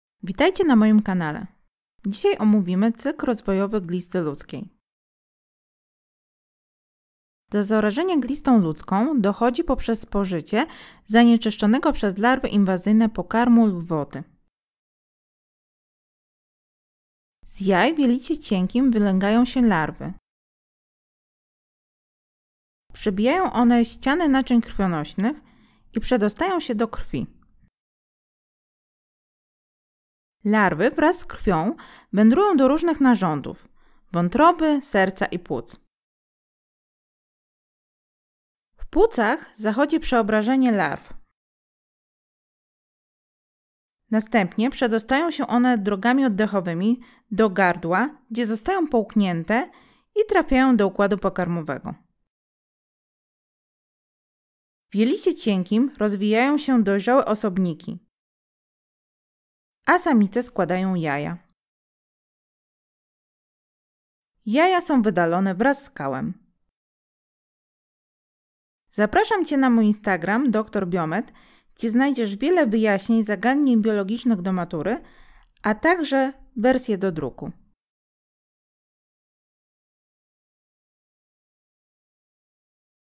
Cykl rozwojowy glisty ludzkiej
Krótka powtórka cyklu rozwojowego glisty ludzkiej.

Zapraszam na mój Instagram @doktor_biomed